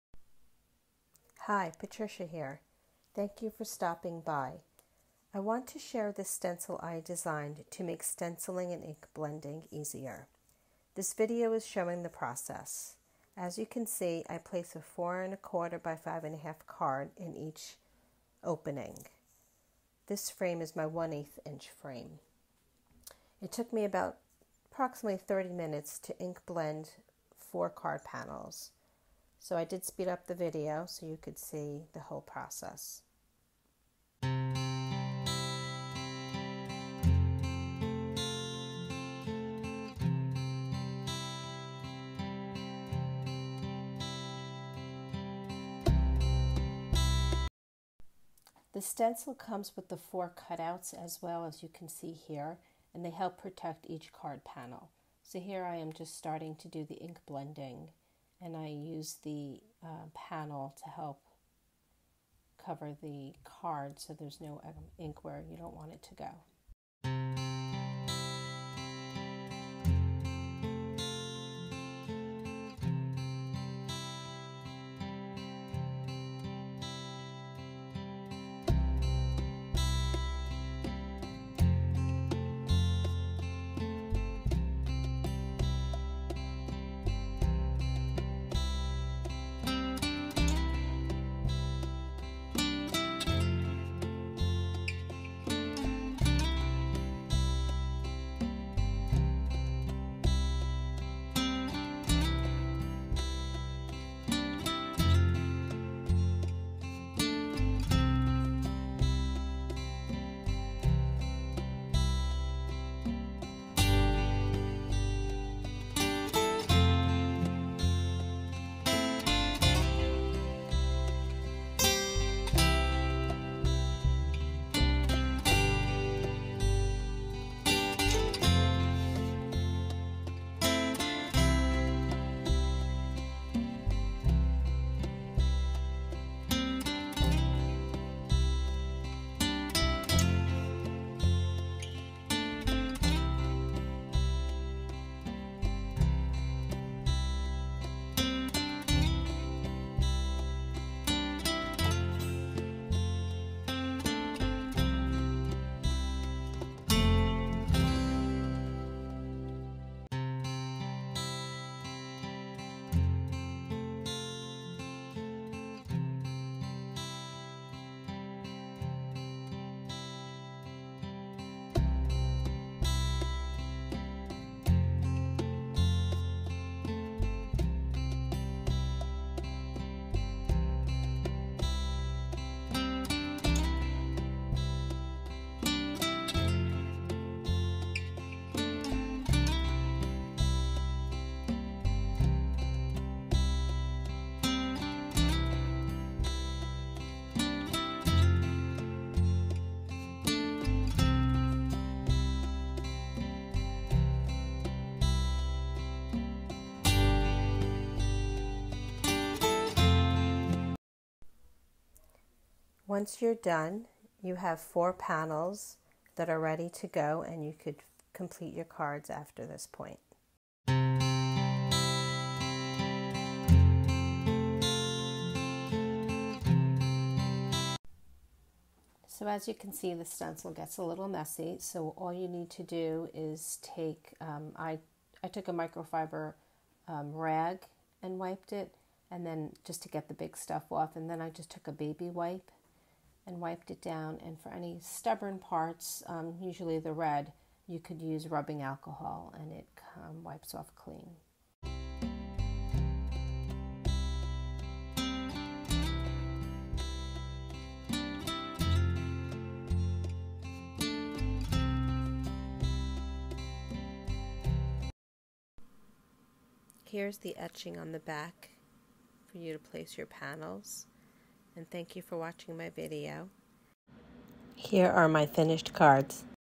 Rainbow cards varied with ink blending- perfect for everyone
Hi. I’m excited to share my 1/8 inch frame stencil that I designed for stenciling and/or ink blending.  This frame helps make the process easier. Place a 4.25 x 5.5 piece of card stock in each of the openings using the etched lines as a guide and you can start stenciling and mass produce up to 4 cards each time.

Watch my video for a quick look at how helpful this stencil can be.

I have two stencil sizes available at PMHDesignsCreations.Etsy.com.

Available for the first time; quantities are small. I will also be adding additional sizes in the future.